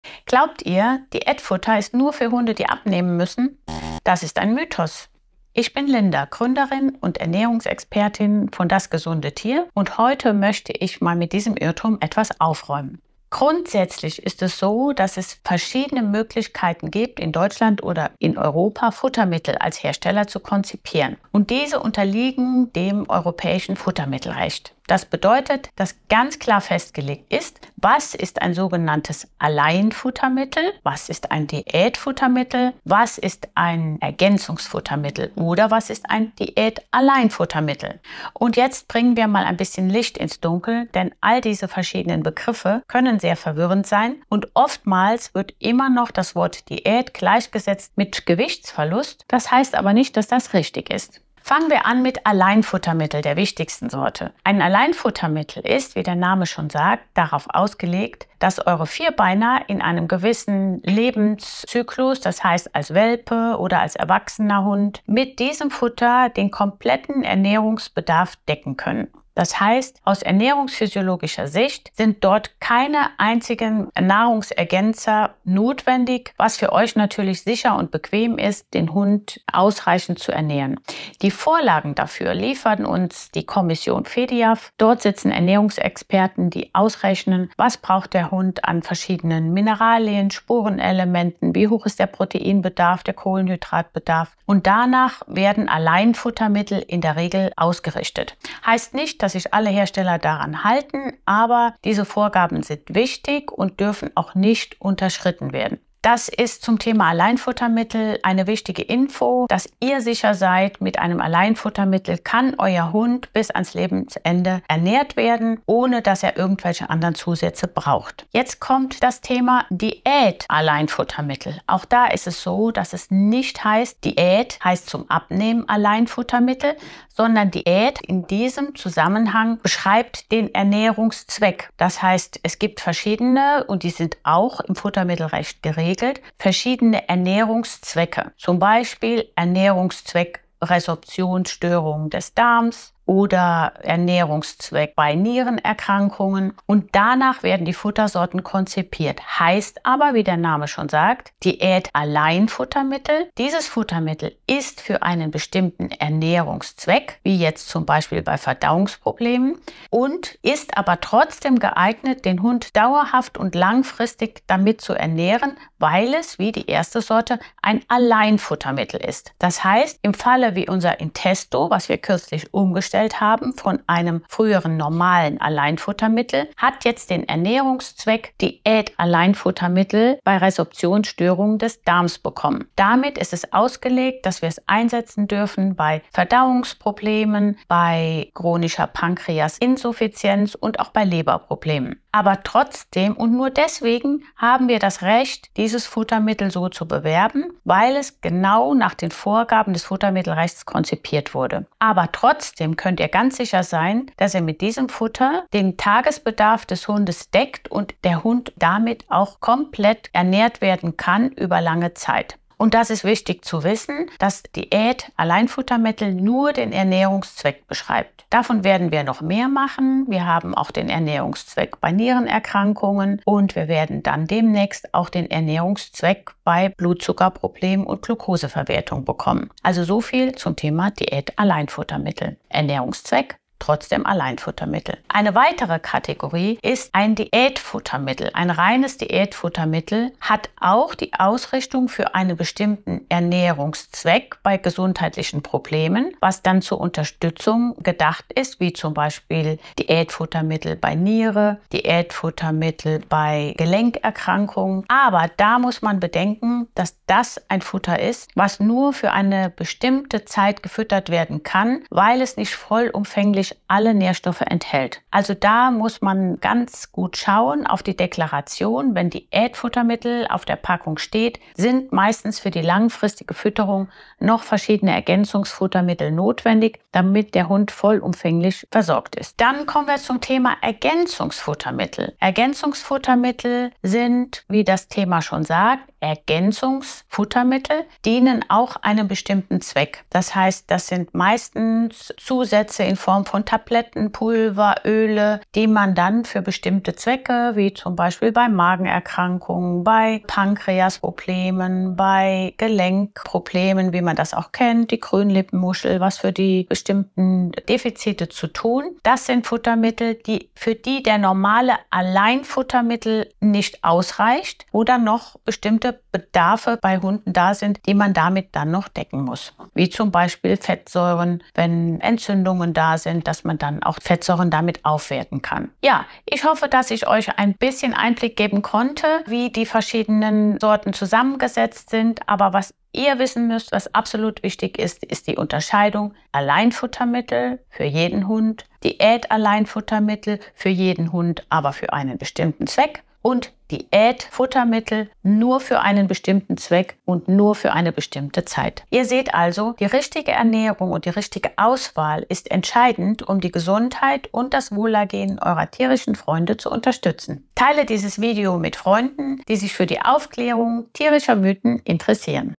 Diätalleinfuttermittel zum Abnehmen füttern!?
Ist Diätfutter wirklich nur für Hunde gedacht, die abnehmen müssen? 🤔 Dieser weit verbreitete Glaube ist tatsächlich ein Mythos! In unserem Video klären wir die Unterschiede zwischen den verschiedenen Futtermittelarten, die auf dem Markt erhältlich sind.

Hier ein Überblick:

Alleinfuttermittel: Diese Futtersorte ist so zusammengesetzt, dass sie alle Nährstoffe liefert, die dein Hund täglich braucht. Du kannst deinem Hund also langfristig ausschließlich dieses Futter geben, ohne dass Mangelerscheinungen auftreten.

Diätalleinfuttermittel: Diese sind speziell für Hunde mit besonderen gesundheitlichen Bedürfnissen entwickelt, wie z. B. für Hunde mit Nierenproblemen oder Übergewicht. Sie unterstützen die Behandlung bestimmter Erkrankungen, sind aber nicht nur auf Gewichtsreduktion ausgelegt. Du kannst diese langfristig füttern und Dein Hund wird mit allem, was er braucht, versorgt sein!

Diätfuttermittel: Oft für spezielle diätetische Bedürfnisse gedacht, können sie in Kombination mit anderen Nahrungsmitteln verwendet werden, um ein bestimmtes Gesundheitsziel zu erreichen, wie z. B. eine bessere Verdauung oder Unterstützung bei Allergien. Diese werden eher kurz- bis mittelfristig gefüttert.

Ergänzungsfuttermittel: Diese Produkte sind dazu gedacht, die Ernährung Deines Hundes zu ergänzen. Sie allein decken nicht den gesamten Nährstoffbedarf ab und sollten daher zusammen mit einem Alleinfuttermittel gefüttert werden.

Entdecke in unserem Video, welche Eigenschaften hinter diesen Bezeichnungen stecken und was sie für die Ernährung Deines Hundes bedeuten!

👉 Erfahre mehr und finde heraus, welches Futter für Deinen Hund das Richtige ist mit unserem Futterberater